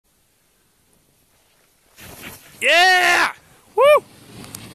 Tim at whitewater.MP4
Whitewater Resort Jan 2011.  This is the new glory ridge side